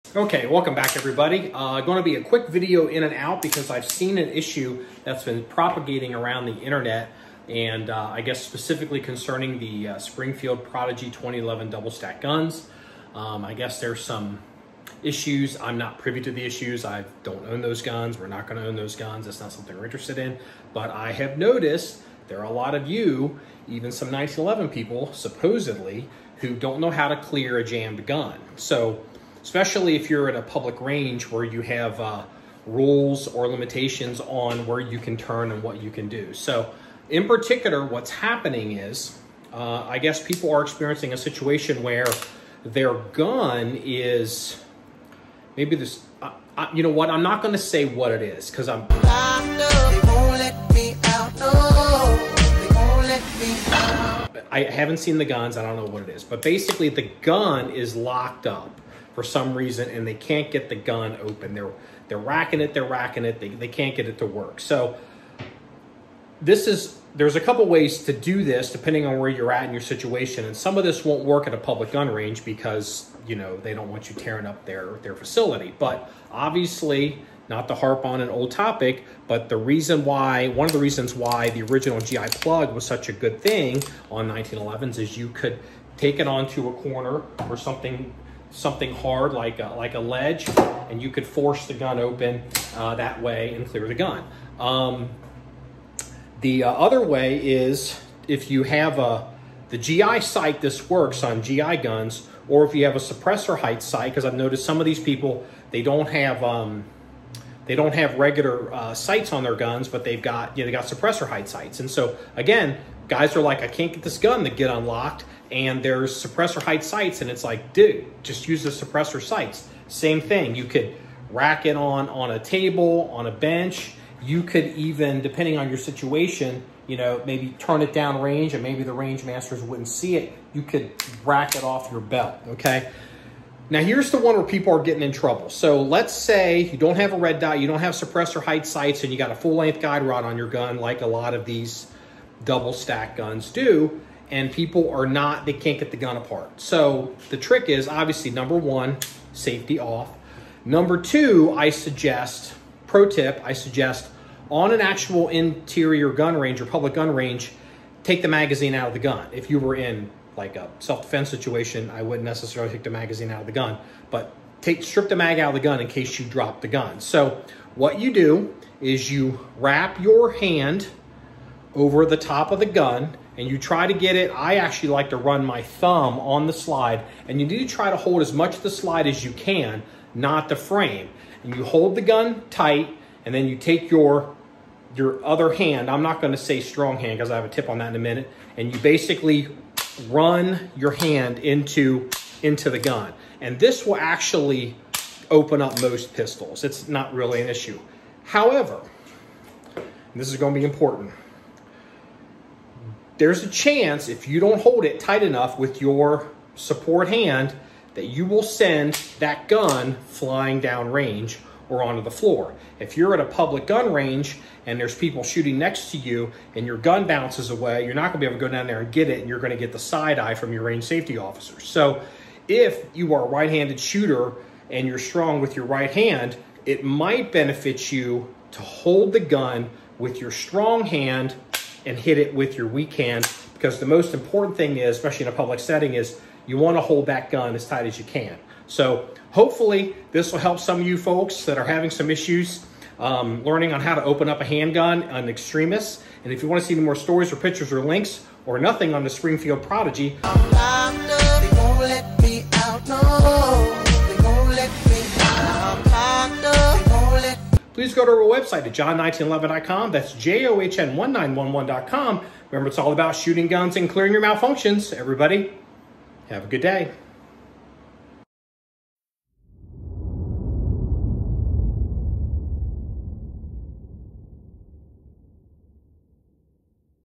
Cracking Open a Locked Pistol
This subject is Basic Pistol 101 stuff. But if you have never been formally trained on pistol manipulations, there is no reason to expect people to know this.

Let me setup the situation. This past week, Springfield Armory released a 2011 format pistol called The Prodigy. Considering the popularity of lower tiered 2011s in the market, it would be silly for Springfield to not make guns in that segment.

However, as with all 1911 pistols, there is no free lunch. Lower tiered pistols will have a number of MIM, and/or, zero fit parts. Considering the volume of guns Springfield makes, it is inevitable there would be individual examples showing problems.

Which is fine. But I have seen a number of people on the internet with guns “locked up” who didn’t seem to have the training tools to solve that problem immediately, on the line.

Note: These techniques cover most guns, not just 1911s or 2011s. Shoot long enough and you’ll see every make of gun locked up. Could be something in the gun is out of spec. Could be the ammunition is out of spec. Could be something inside the gun is broken.

Barring a catastrophic failure of some kind that would require the gun being cut open, I don’t know a single gunsmith or manufacturer that will accept a locked up gun with a loaded round in the chamber. At least not through the mail.

So you gotta get the live round out of the gun. This is where you start.

Also…in a self defense / working gun situation, this is how you get the errant round out of the gun in a gunfight / match. After which hope & pray the rest of the magazine runs fine.

Sincerely,
Marky

Amazon - Audible